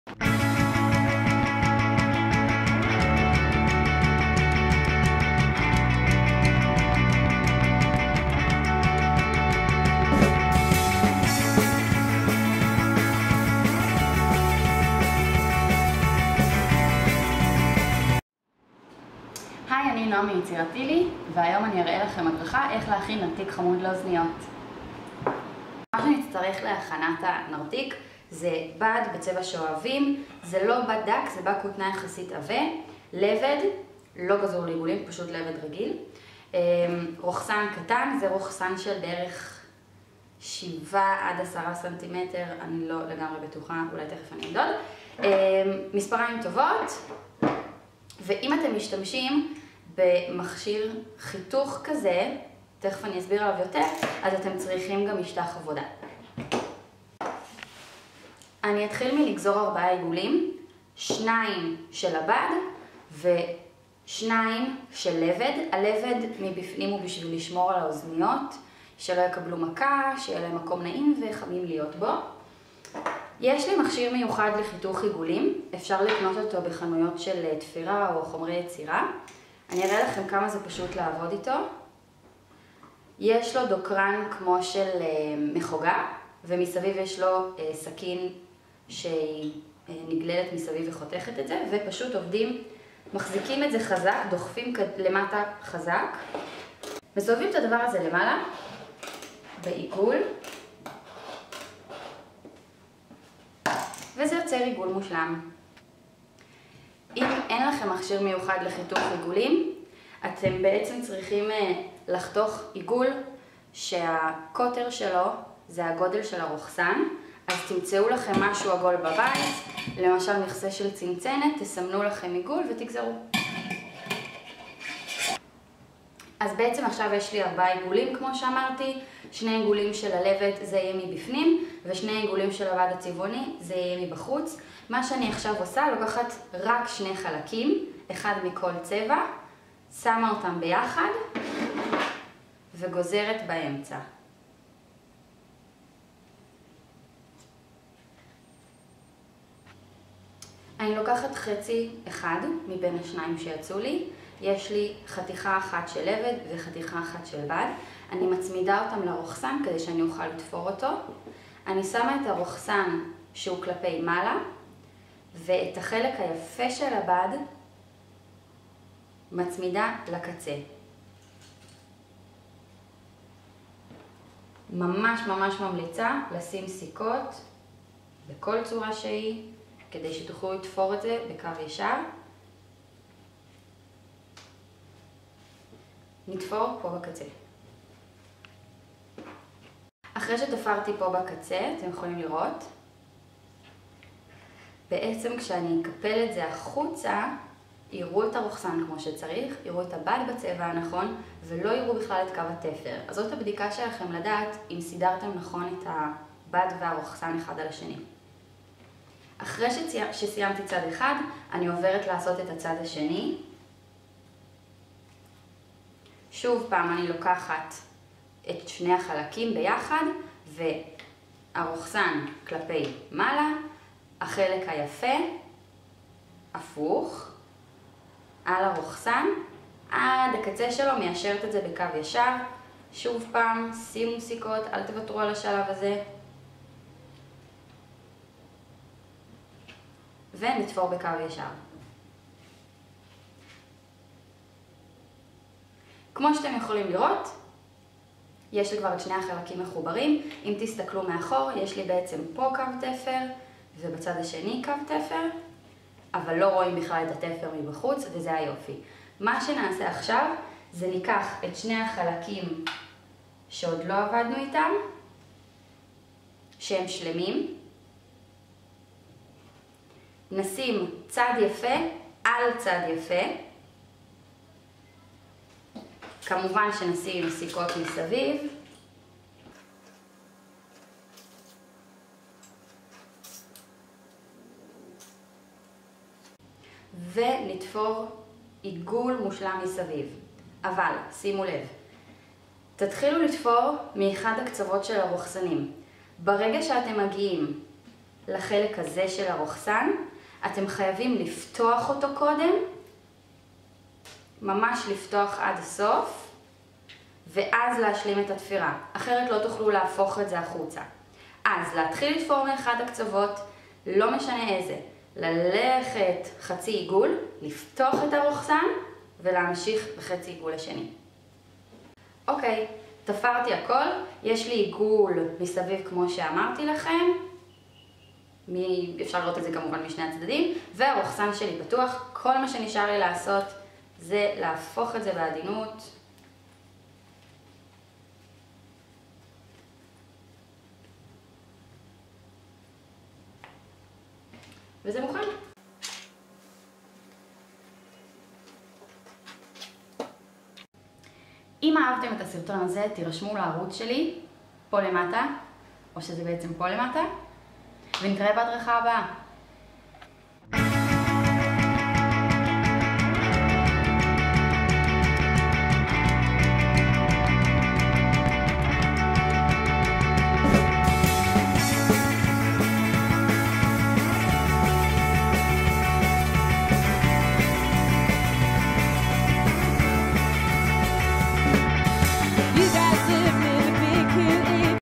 איך לתפור נרתיק לאוזניות | sewing a headphone case
הדרכה מבית "יצירתילי". בהדרכה הזו אני מראה איך לתפור נרתיק לכיסוי אוזניות. מתאים לתפירה למתחילים ומתחילות.
ניתן לרכוש ערכת תפירה בדיוק לפי הסרטון ובמגוון צבעים באתר שלי! עכשיו רק ב-20 ש"ח!